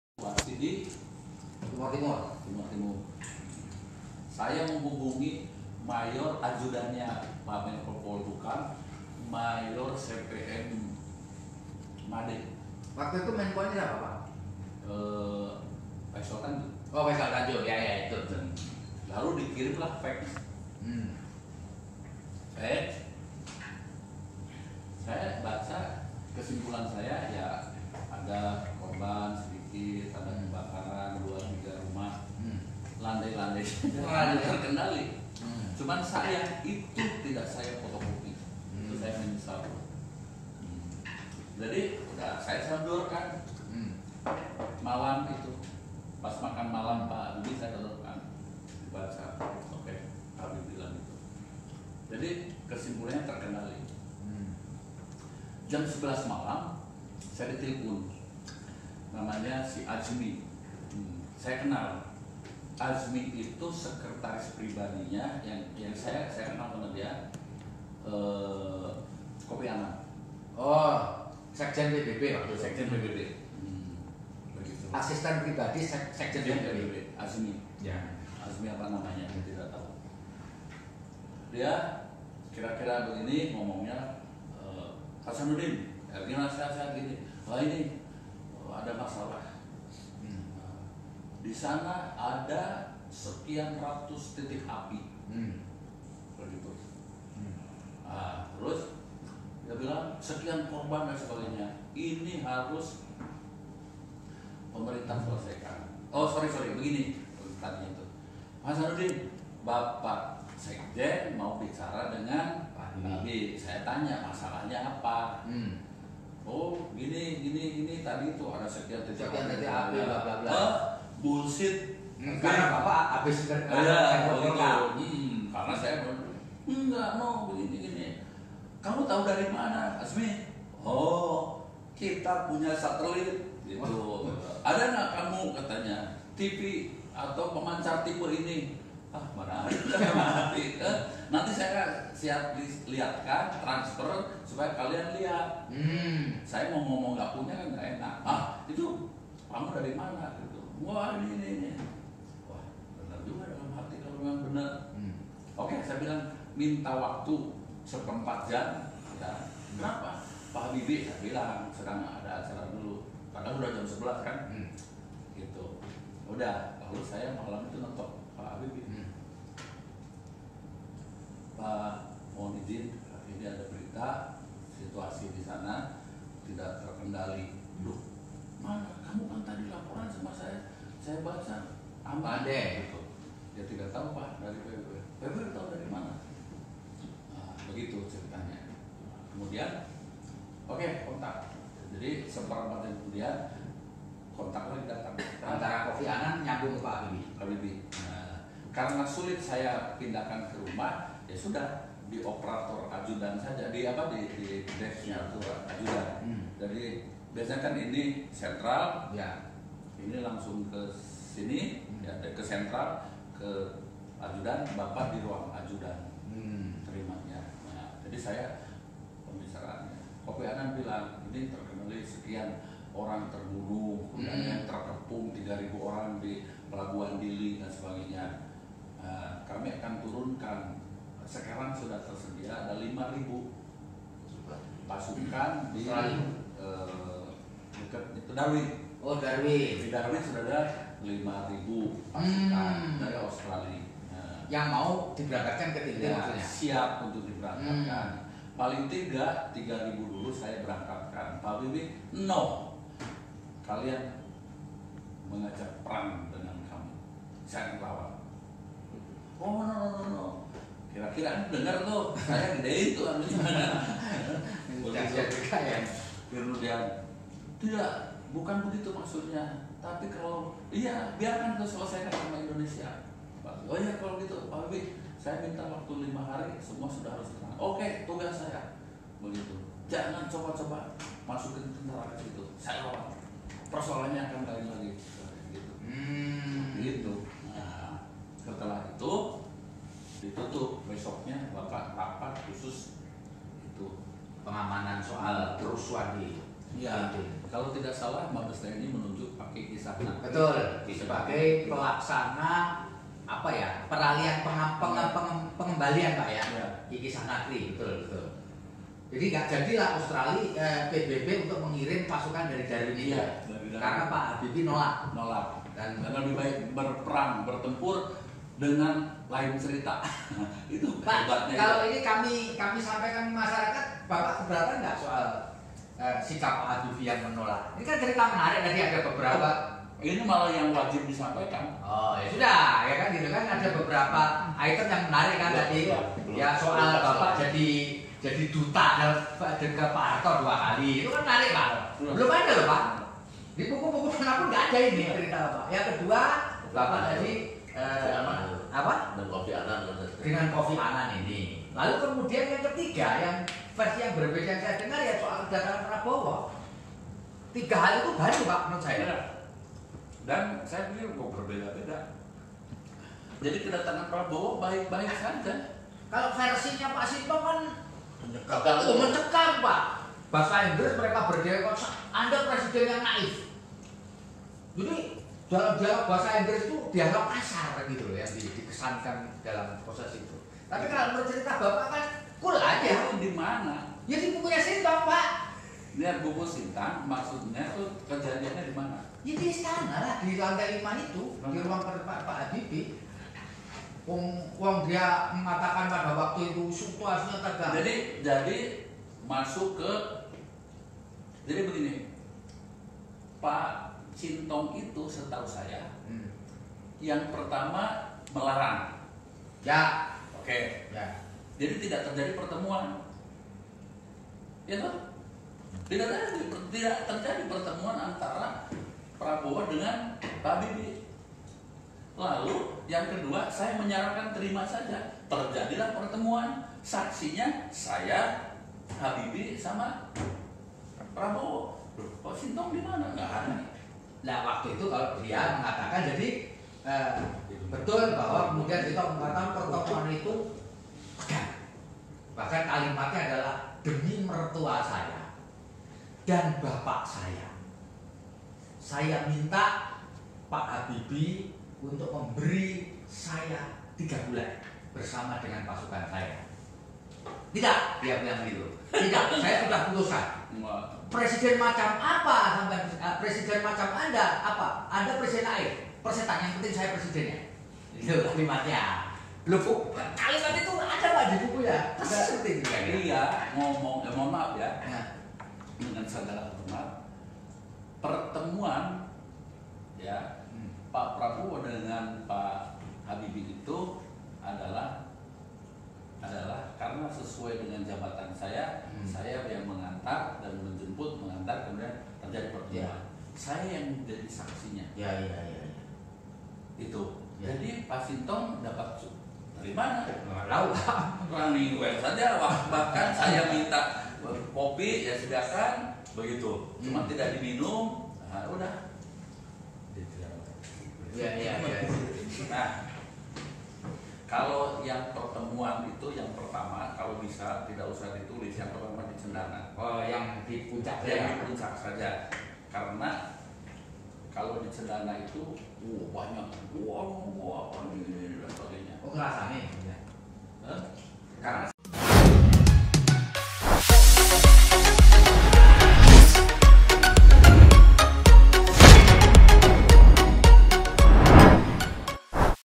Kisah TB Hasanuddin, Mantan Ajudan BJ Habibie (2)
TRIBUNNEWSWIKI.COM - Mayor Jenderal (Purn) Tubagus (TB) Hasanuddin, mantan ajudan Presiden Ketiga Indonesia, Prof Dr Ir Ing Bacharuddin Jusuf (BJ) Habibie, punya kisah yang selalu dikenangnya.

Dalam wawancara di Kantor Tribun Network, Jakarta Pusat, Jumat (13/9/2019) pagi, TB Hasanuddin berkisah sejumlah cerita menarik saat mendampingi Habibie pada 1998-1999.

Terlebih pada saat itu, Habibie menjadi presiden saat situasi di Indonesia sedang kacau.

"Waktu itu ada banyak informasi, ada pasukan liar, ada jenderal yang datang, yang tentu saja harus saya atasi dengan baik-baik, harus saya berikan informasi," ujar TB Hasanuddin, Rabu (11/9/2019).

Editor: TRIBUNTIMUR/HARDIANSYAH